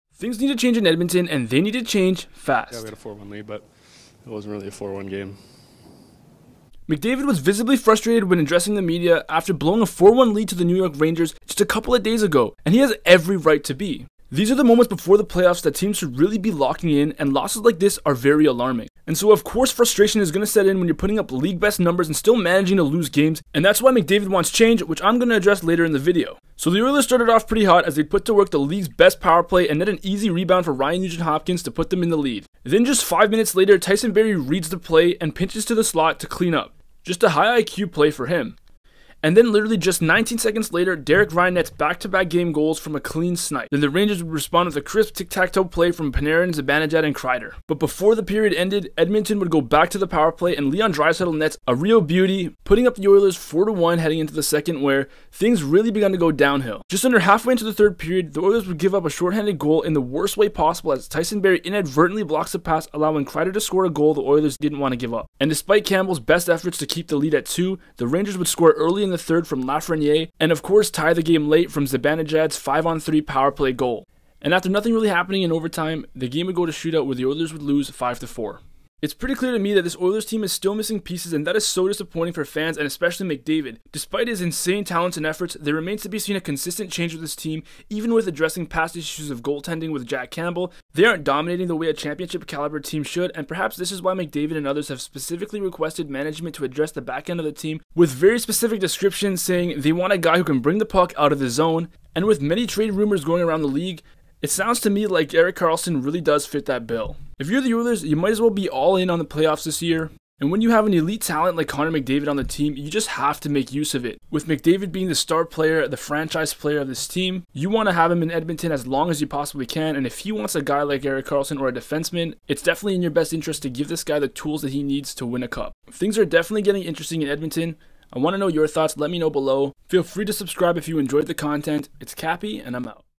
Connor Mcdavid wants THIS to happen RIGHT NOW...
Connor Mcdavid has achieved everything in hockey at the young age of 26 EXCEPT for a Stanley Cup. He's had little luck on his quest despite dominating the NHL which has become very frustrating. As he prepares to take another shot at Lord Stanley, he recognizes things need to change in order for his team to have success.